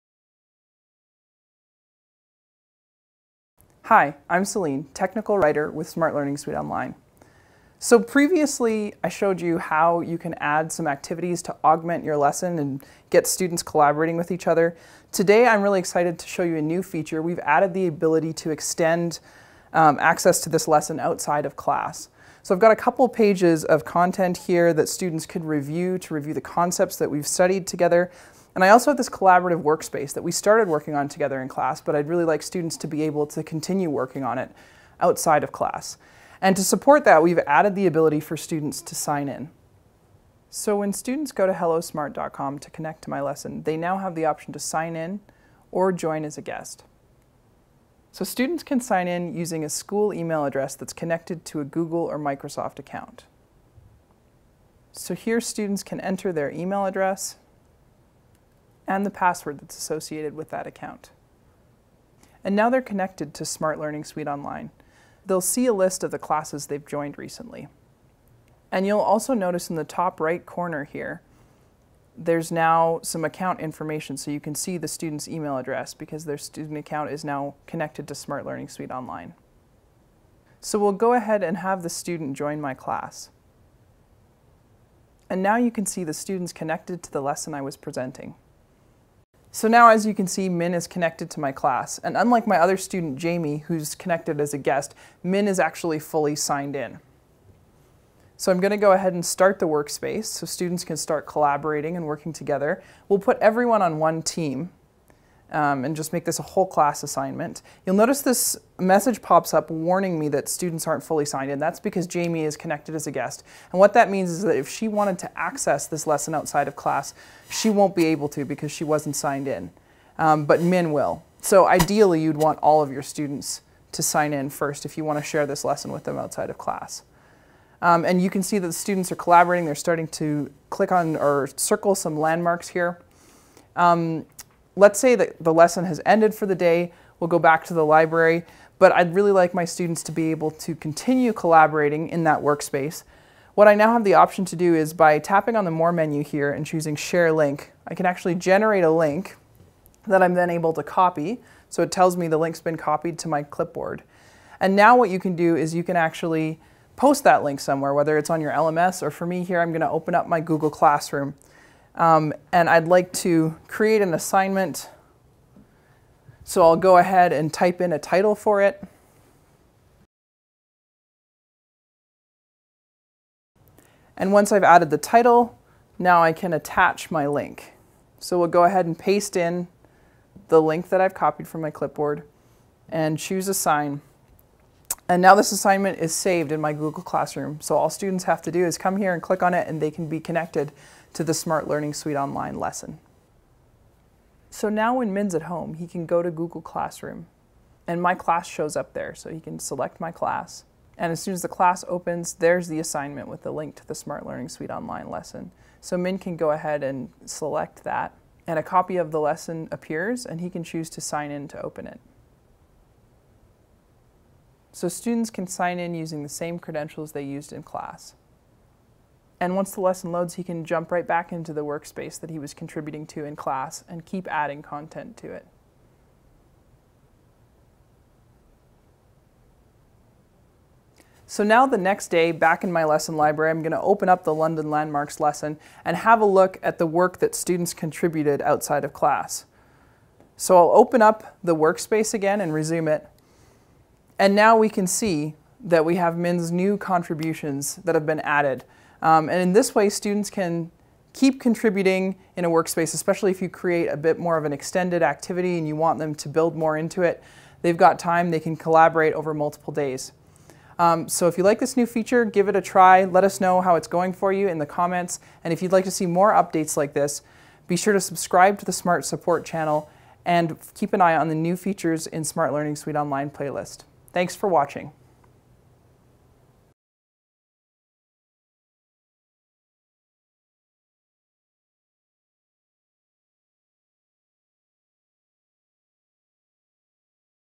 Accessing lessons outside of class in Lumio by SMART (January 2019)
Have students sign into Lumio (formerly SMART Learning Suite Online) so that they can access a lesson outside of class when you share a student access link. Students with a link can review the lesson and continue workspace collaboration outside of class.